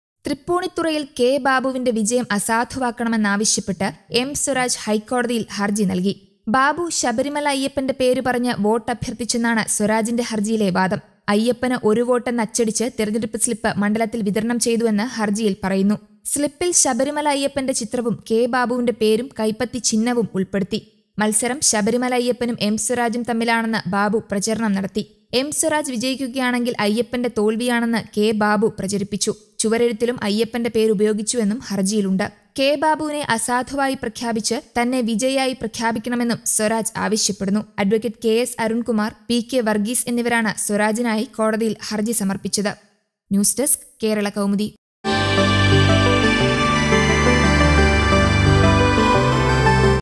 M Swaraj files petition in HC seeking annulment of K Babu's victory | Keralakaumudi
തൃപ്പൂണിത്തുറയില്‍ കെ ബാബുവിന്‍െ  വിജയം  അസാധുവാക്കണമെന്ന് ആവശ്യപ്പെട്ട് എം സ്വരാജ് ഹൈക്കോടതിയില്‍ ഹര്‍ജി നല്‍കി. ബാബു ശബരിമല അയ്യപ്പന്റെ പേര് പറഞ്ഞ് വോട്ട് അഭ്യര്‍ത്ഥിച്ചെന്നാണ് സ്വരാജിന്റെ ഹര്‍ജിയിലെ വാദം.
അയ്യപ്പന് ഒരു വോട്ട് എന്ന് അച്ചടിച്ച് തെരഞ്ഞെടുപ്പ് സ്ലിപ്പ്  മണ്ഡലത്തില്‍ വിതരണം ചെയ്തു എന്ന് ഹര്‍ജിയില്‍ പറയുന്നു.  സ്ലിപ്പില്‍ ശബരിമല അയ്യപ്പന്റെ ചിത്രവും  കെ ബാബുവിന്റെ പേരും കൈപ്പത്തി ചിഹ്നവും ഉള്‍പ്പെടുത്തി. മത്സരം ശബരിമല അയ്യപ്പനും എം സ്വരാജ് തമ്മില്‍ ആണെന്ന്  ബാബു പ്രചരണം നടത്തി.